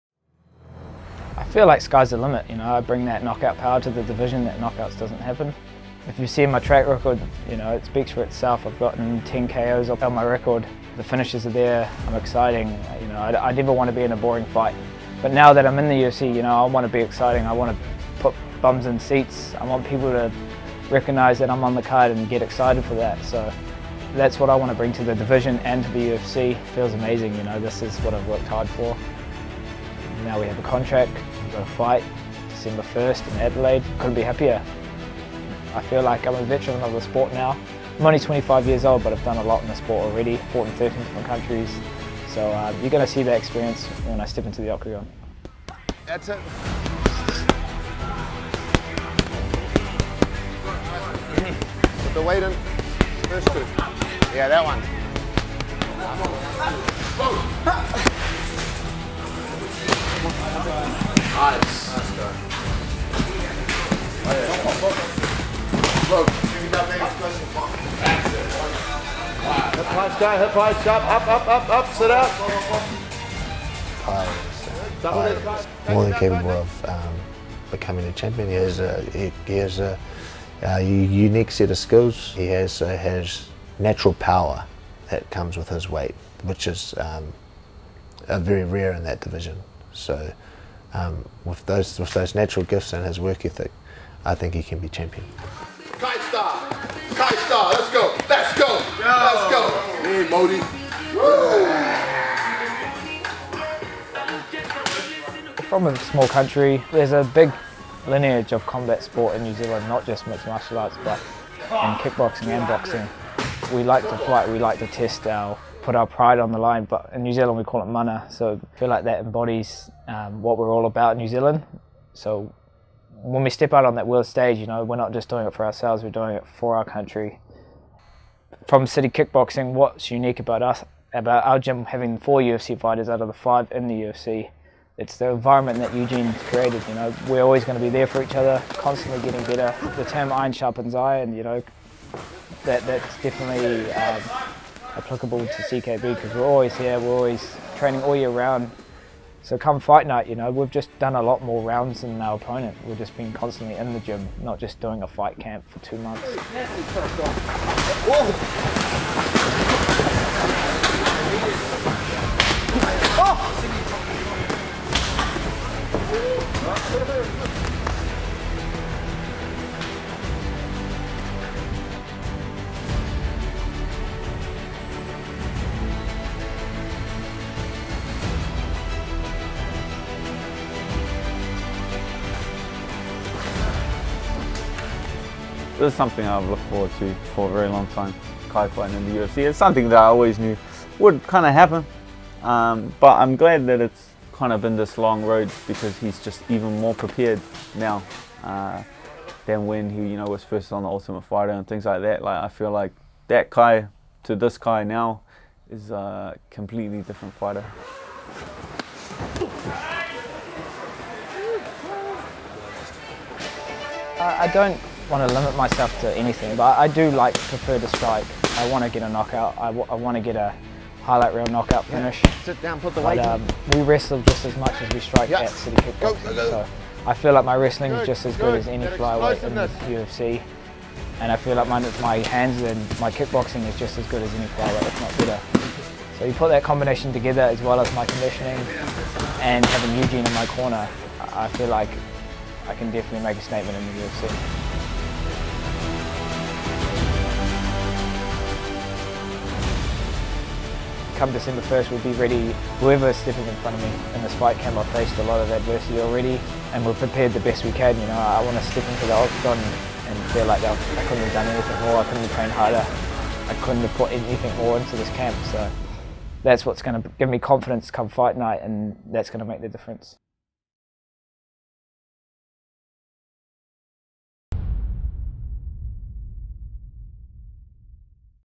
Kai "Don't Blink" Kara-France UFC Debut
Rising New Zealand flyweight prospect, Kai "Don't Blink" Kara France, has been signed to the UFC amassing a 17-7 record with a 5-fight win streak and winning 10 out of his last 11 bouts.
Kara-France will be making his debut against Elias Garcia.
The card  features fellow Kiwi Mark Hunt, in his final fight for the UFC after eight years with the organization and the show is headlined by former UFC heavyweight champion Junior dos Santos as he faces Australian Tai Tuivasa in the main event in Adelaide on December 1st.